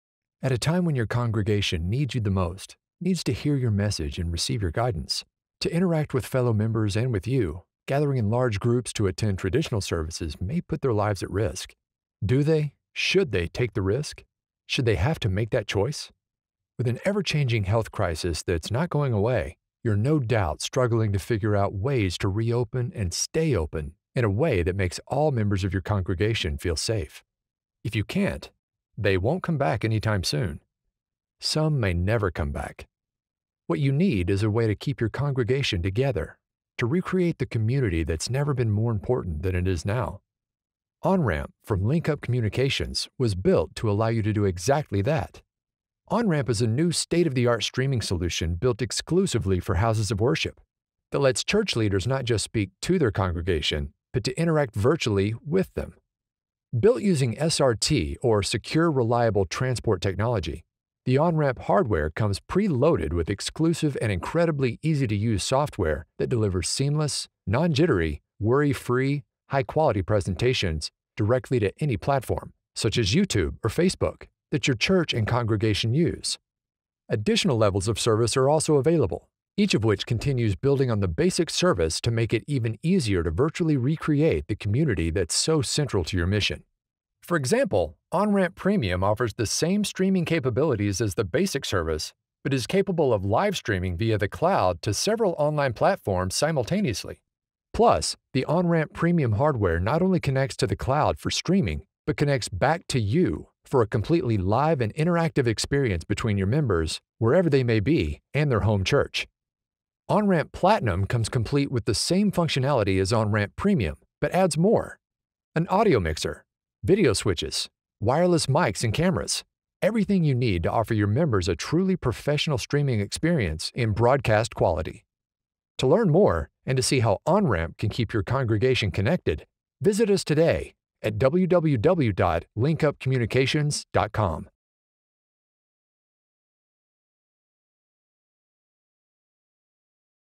OnRamp for Houses of Worship from LinkUp and International Datacasting
Check out our explainer video about OnRamp - a new, state-of-the-art streaming solution built exclusively for houses of worship.

OnRamp not only provides an affordable path for church leaders to speak to their congregation, but to interact virtually WITH them, as well.

OnRamp connects to the cloud for streaming, then connects BACK to the host for a completely live and interactive experience.